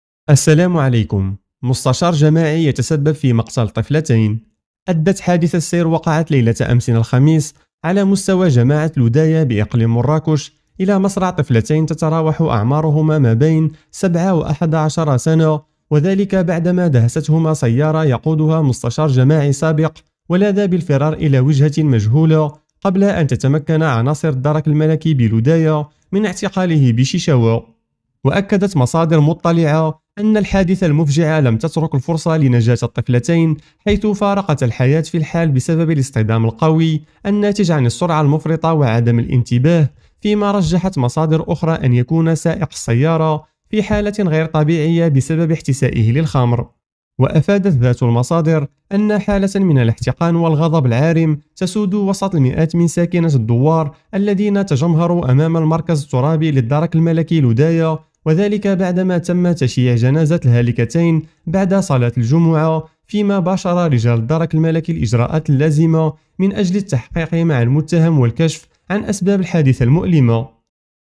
مستشار جماعي دهس طفلتين و هرب أخبار المغرب اليوم على القناة الثانية دوزيم 2M
شوفو شنو دار مسؤول مغربي كبير و هرب أخبار المغرب اليوم على القناة الثانية دوزيم 2M
أدت حادثة سير وقعت ليلة أمس الخميس، على مستوى دوار اولاد بن السبع بجماعة الأوداية على تراب إقليم مراكش، إلى مص.رع طفلتين تتراوح أعمارهما مابين 7 و11 سنة، وذلك بعدما دهست.هما سيارة يقودها مستشار جماعي سابق بجماعة المزوضية، ولاذ بالفرار إلى وجهة مجهولة قبل أن تتمكن عناصر الدرك الملكي بالوداية من القبض عليه بشيشاوة.
وأكدت مصادر مطلعة أن الحادثة المف.جعة لم تترك الفرصة لنج.اة الطفلتين، حيث فار قتا الحياة في الحال بسبب الاص.طدام القوي الناتج عن السرعة المفرطة وعدم الانتباه، فيما رجحت مصادرنا أن يكون سائق السيارة في حالة غ ير طب يعية بسبب احتساءه للخ مر.
وأفادت ذات مصادر بأن حالة من الاحتقان والغضب العارم تسود وسط المئات من ساكنة الدوار الذين تجمهروا أمام المركز الترابي للدرك الملكي الاوداية، وذلك بعدما تم تشيع جنازة الهالك.تين بعد صلاة الجمعة، فيما باشر رجال الدرك الملكي الاجراءات اللازمة من أجل التحقيق مع المتهم والكشف عن أسباب الح ادثة المؤل مة.